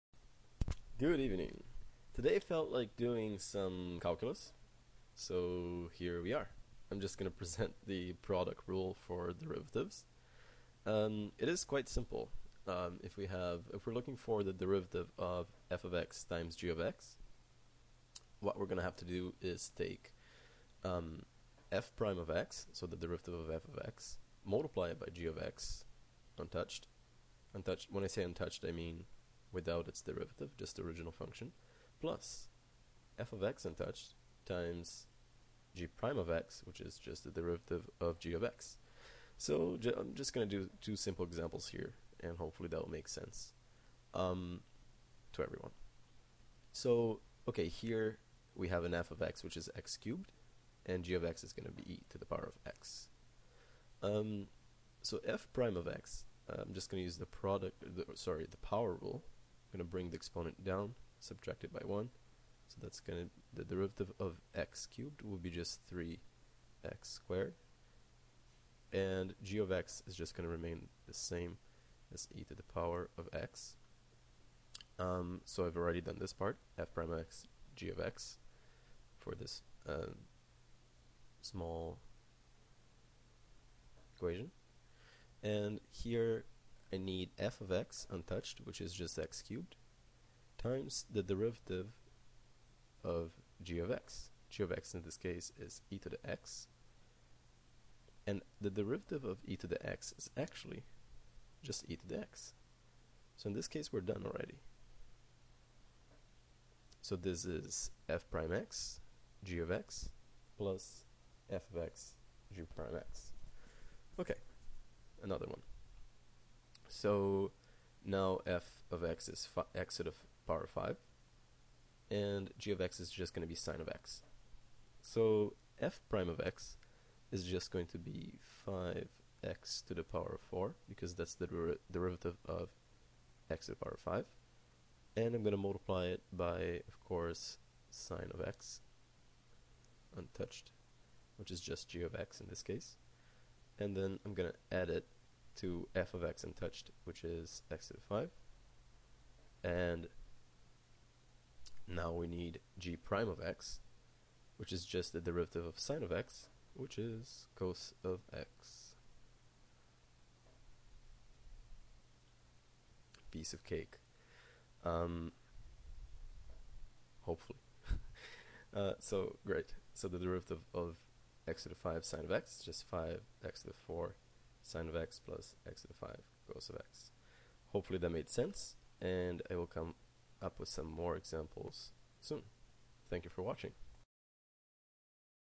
Derivatives - Product Rule (Calculus) -EASY Explanation!!!!
Two simple examples of the product rule.